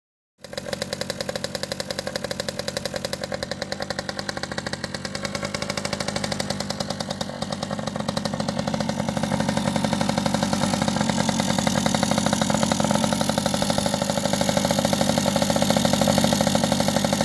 1979 Yamaha 100cc Cafe Racer
First start up of my just completed custom built 1979 Yamaha 100cc Cafe Racer.  It was a DT100 in it's past life.